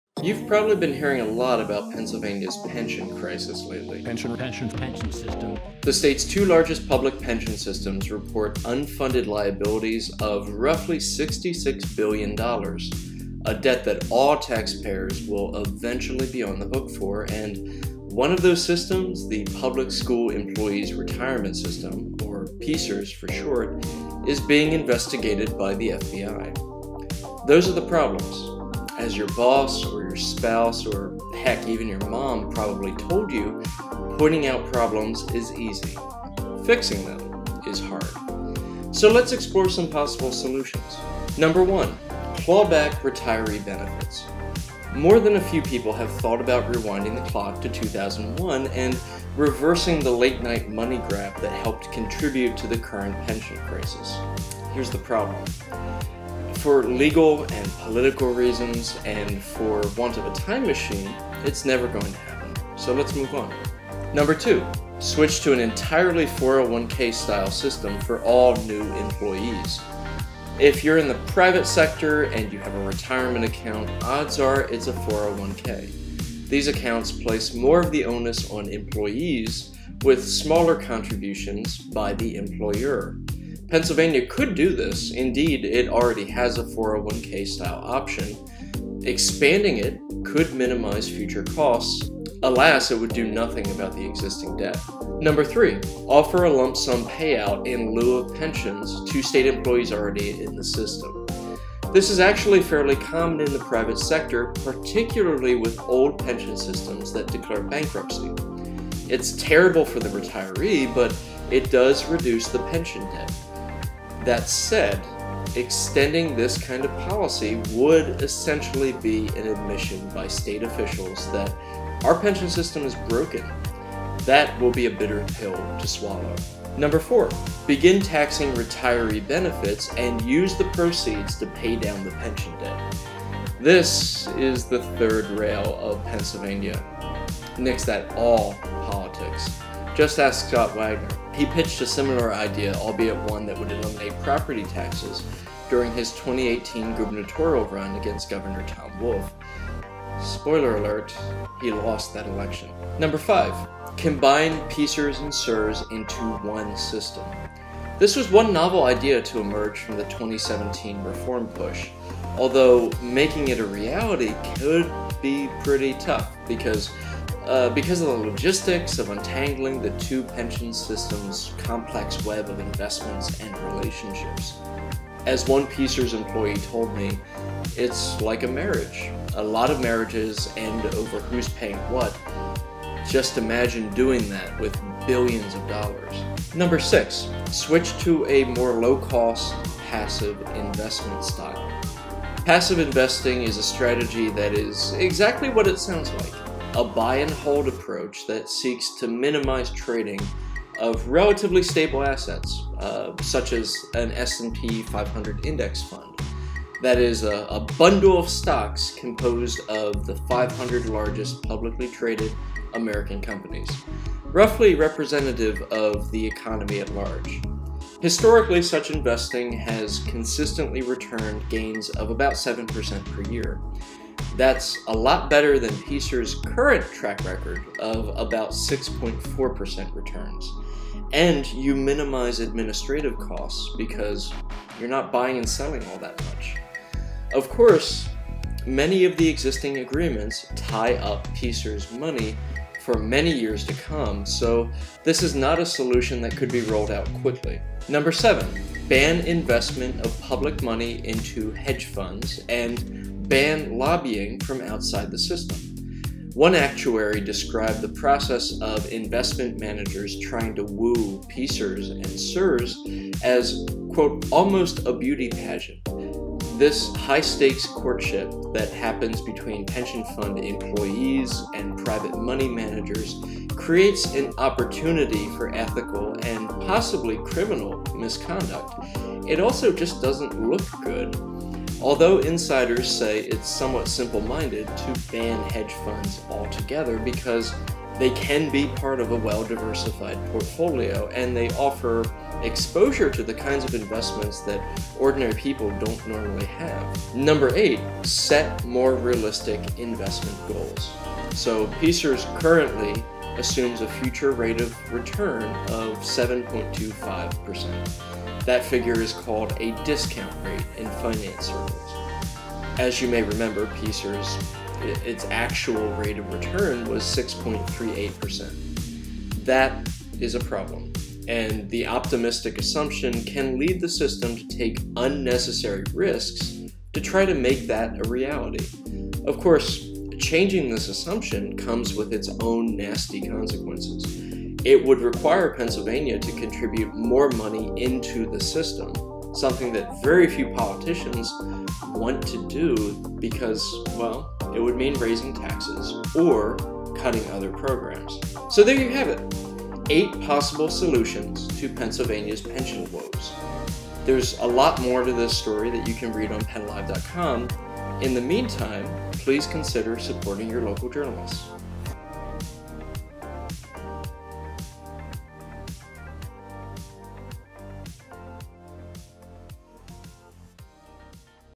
The $66 billion question: How can we fix Pa.’s pension crisis?
You've heard all about the issues surrounding Pennsylvania's largest public pension funds. Now: How do we fix them?